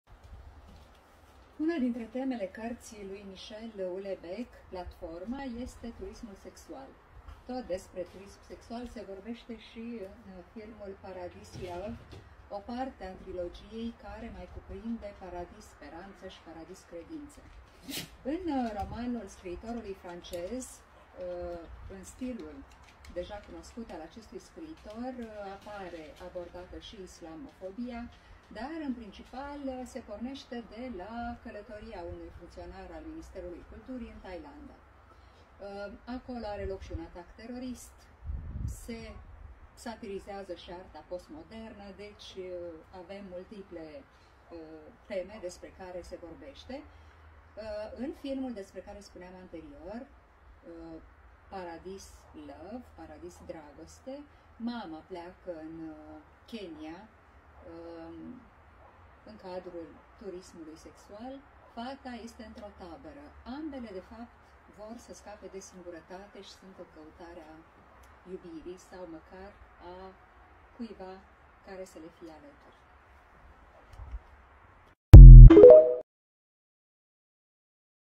"Platforma", de Michel Houellebecq & "Paradise: Love"- film
Despre turismul sexual într-un roman francez contemporan și în filmul "Paradis: Dragoste"